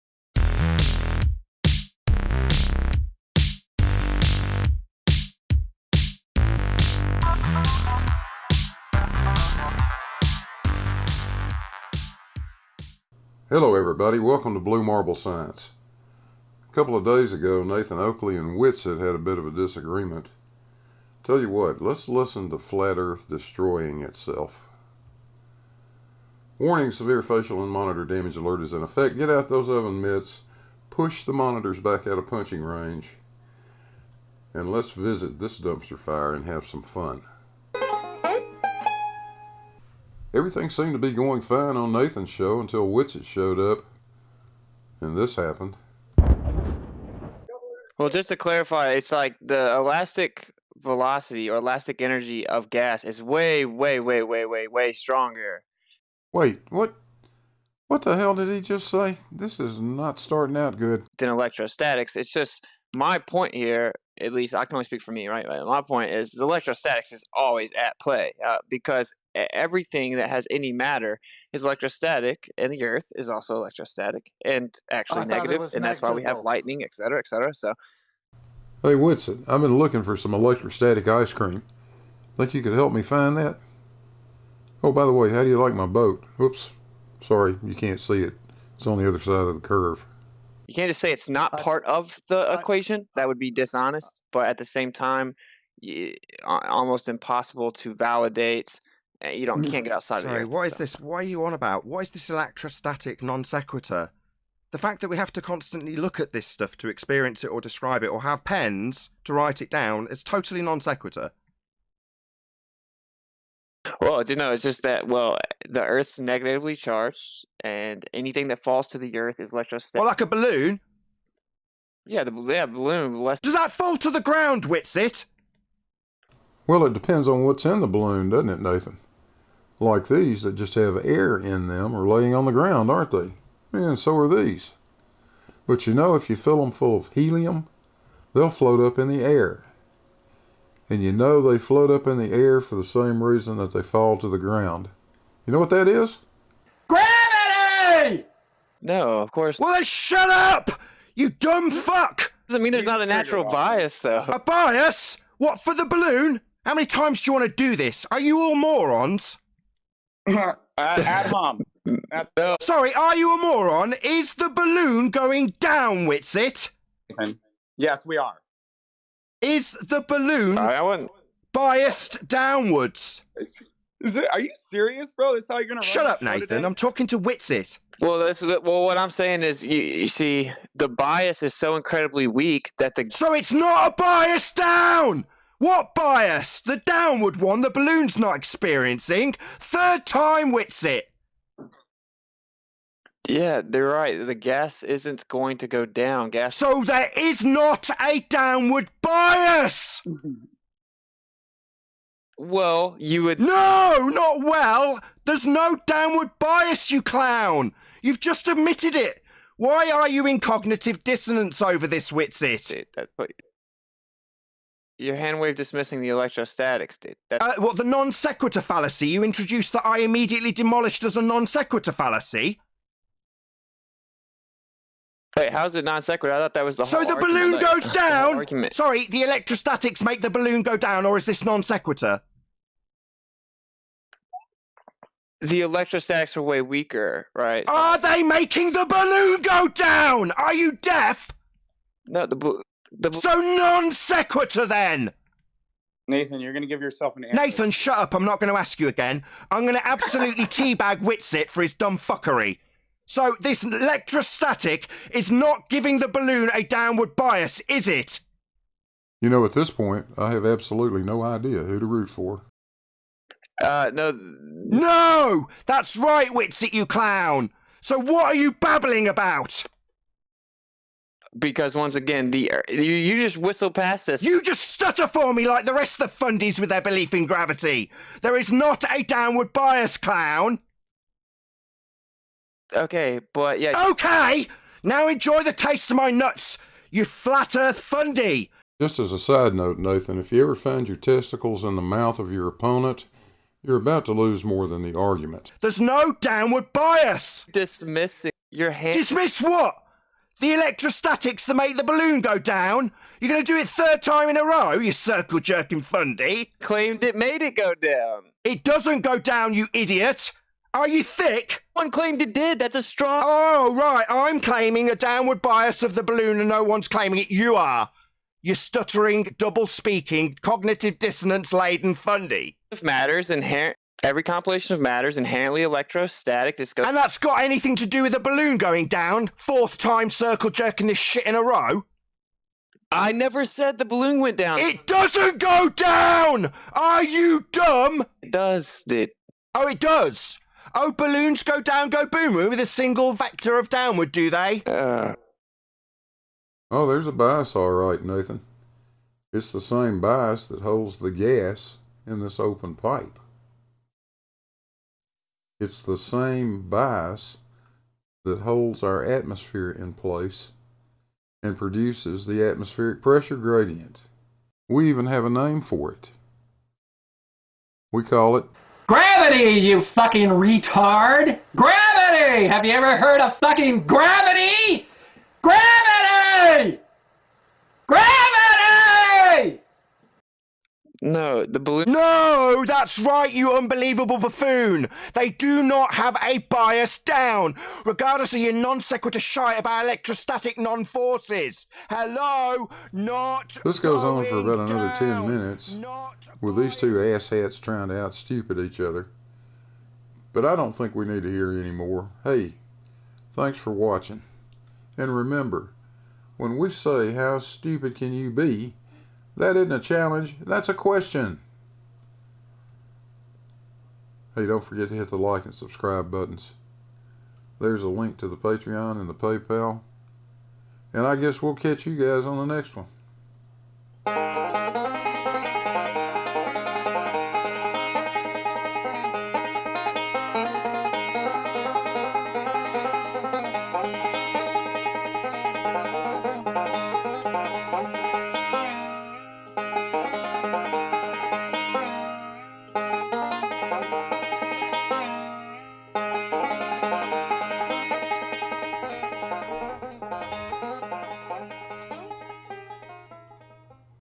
Nathan Oakley vs Witsit
Witsit seems to think electrostatic forces explain that inconvenient downward acceleration we know to be gravity. Nathan isn't ammused!

You can now join the BMS channel – click the “Join” button above and check it out!